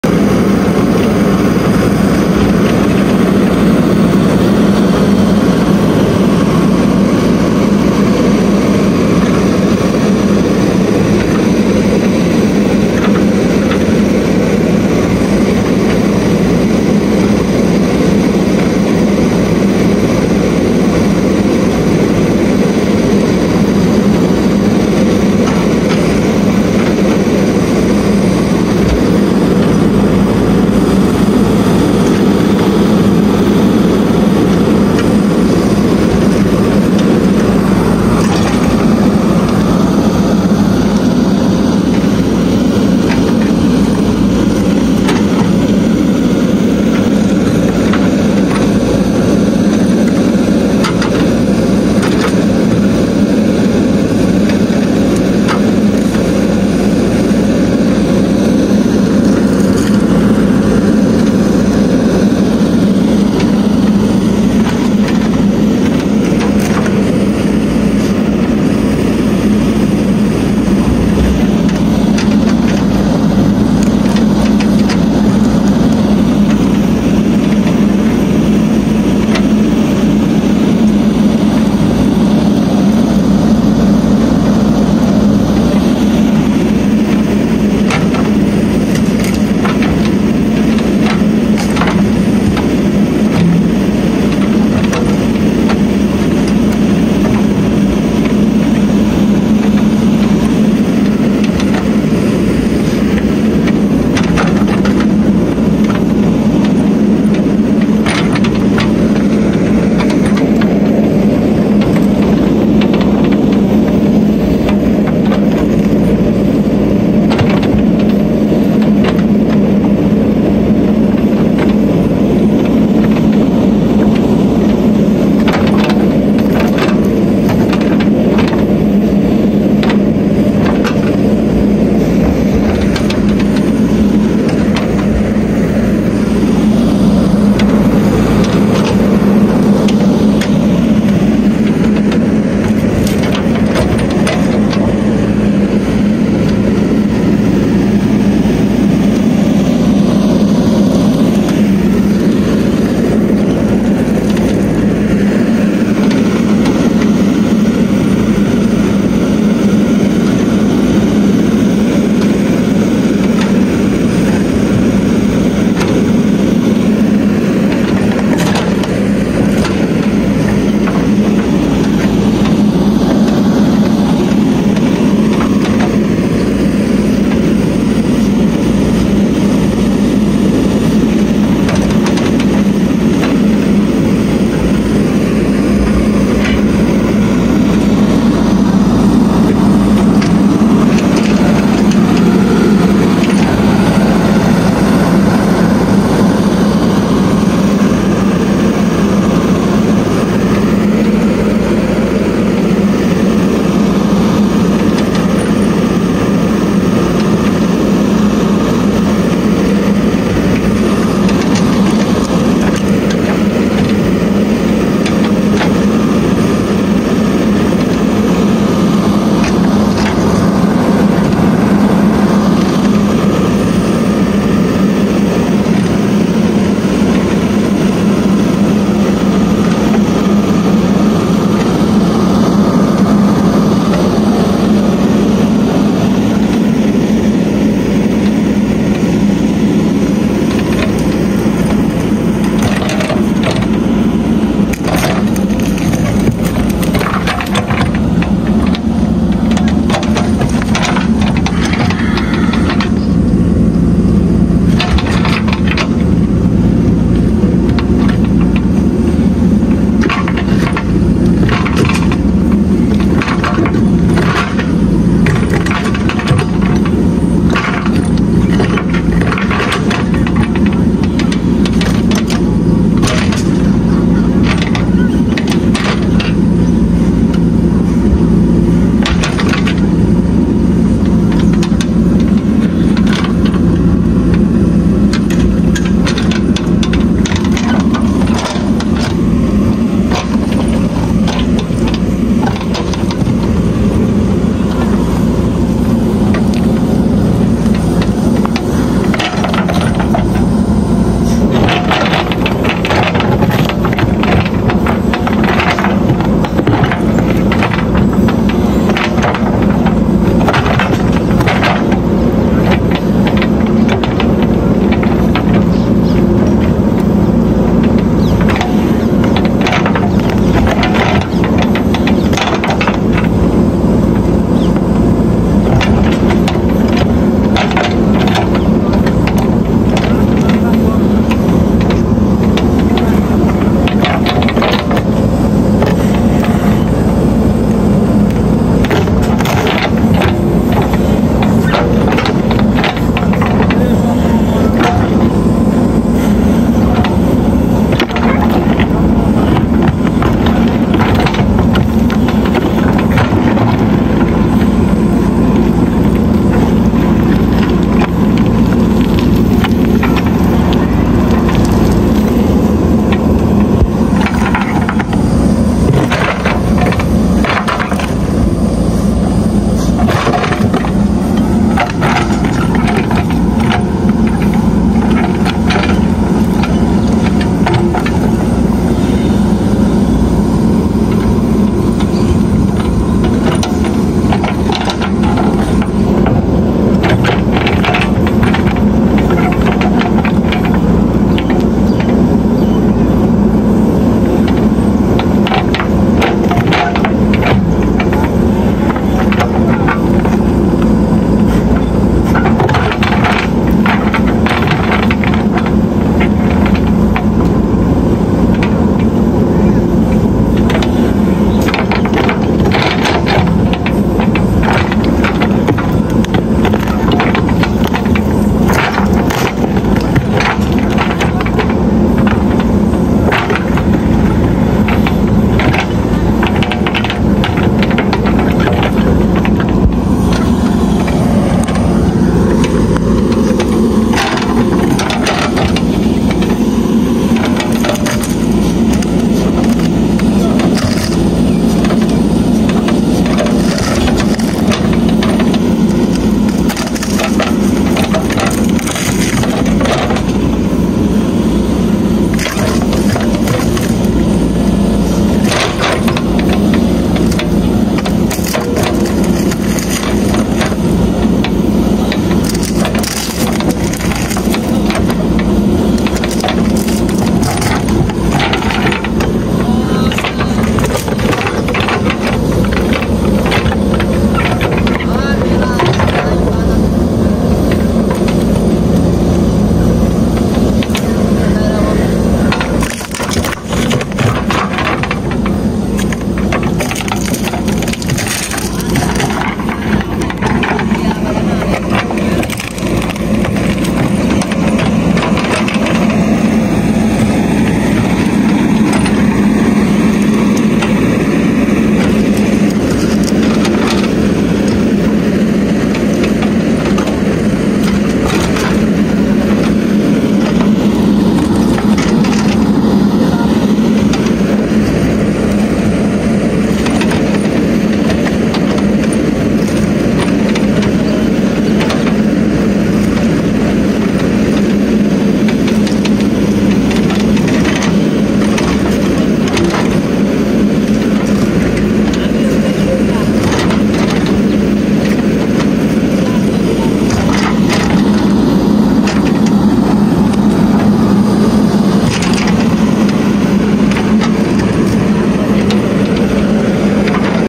Roots Of Bamboo Are Being Uprooted For The Construction Of The Tunnel
In this video, bamboo roots are being uprooted for the construction of a tunnel. The JCB machine is active after the youth of the village went abroad. Because a skilled operator is doing the job. Due to which the villagers are happy. This is what I informed.